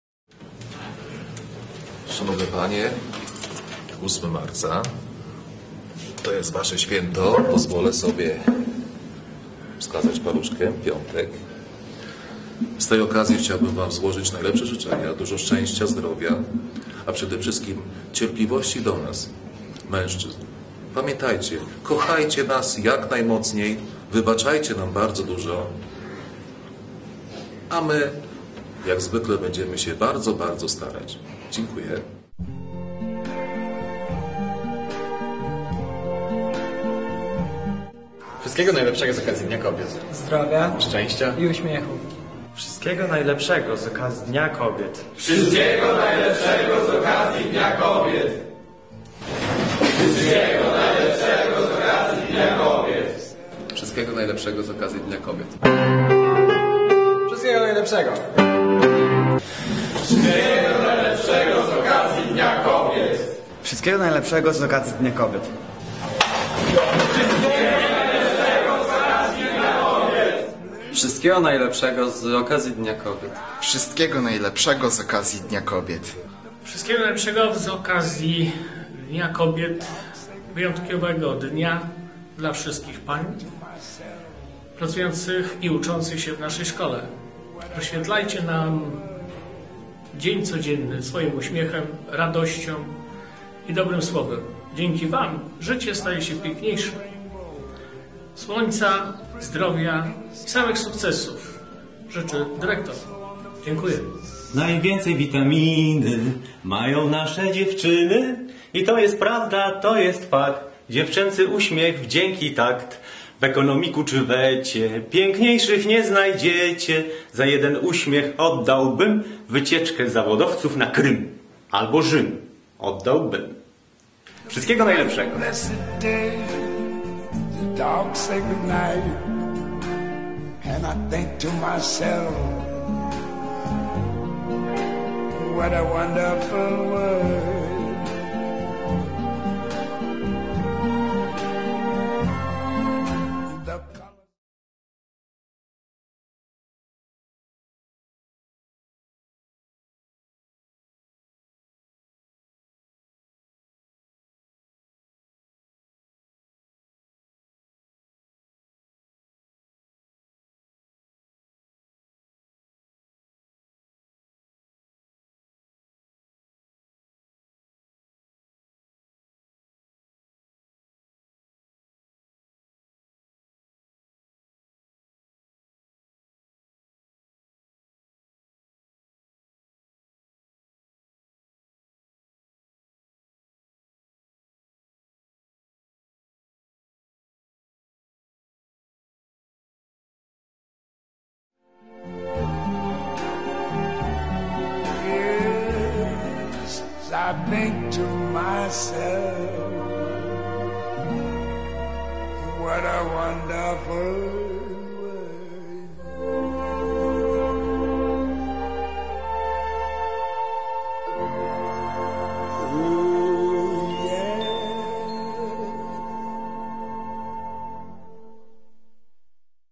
Dzień Kobiet
Z okazji Dnia Kobiet
Tyle spokoju, ile tylko potrzebujecie,
Tyle szaleństwa, aby życie nie wydawało się nudne,
Tyle szczęścia, abyście nie oszalały z nadmiaru,
zaś miłości ponad wszystko
życzą Faceci z Mechaniaka